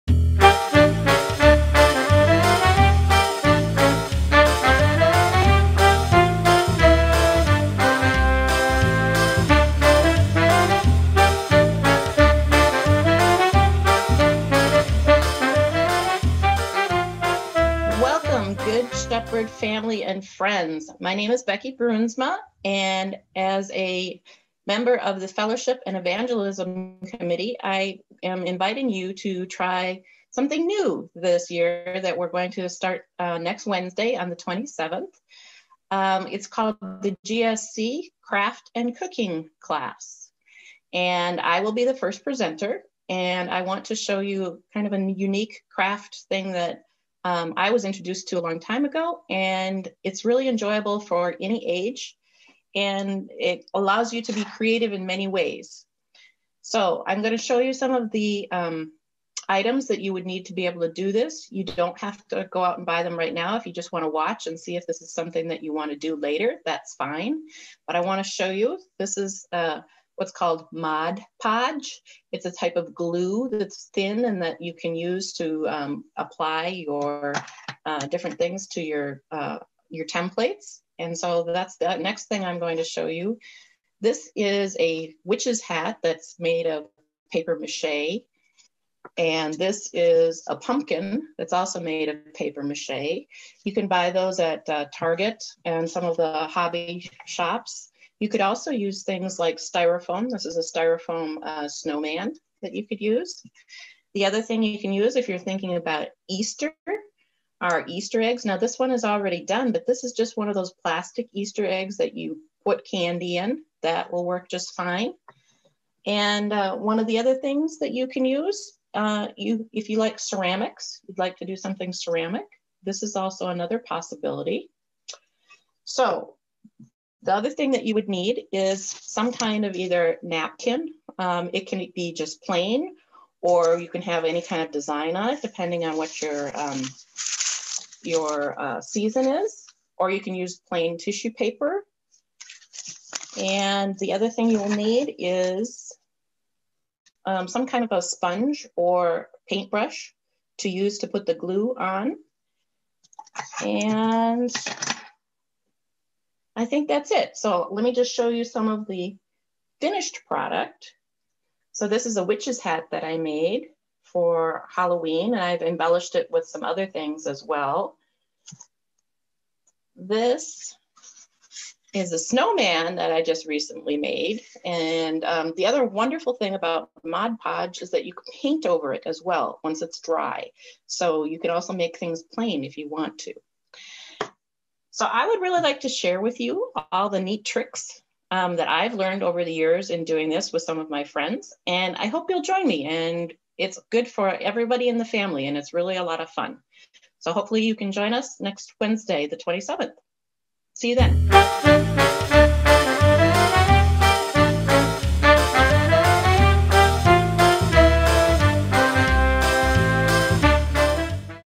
The Great Good Shepherd Craft and Cooking Class: Jan 27 Promo
begins on Wednesday, Jan. 27 @ 7pm via Zoom. Becky Bruinsma will be demonstrating how to use Mod Podge for a variety of fun craft projects that are family friendly for all skill levels. Items needed for Mod Podge class include either gloss or matte Mod Podge glue (Target, Walmart, Michaels, Hobby Lobby), small sponge brush or paint brush, napkins or tissue paper to apply to your paper mache, ceramic, wood, or styrofoam form (online or same stores above), your sense of humor and fun creative mind!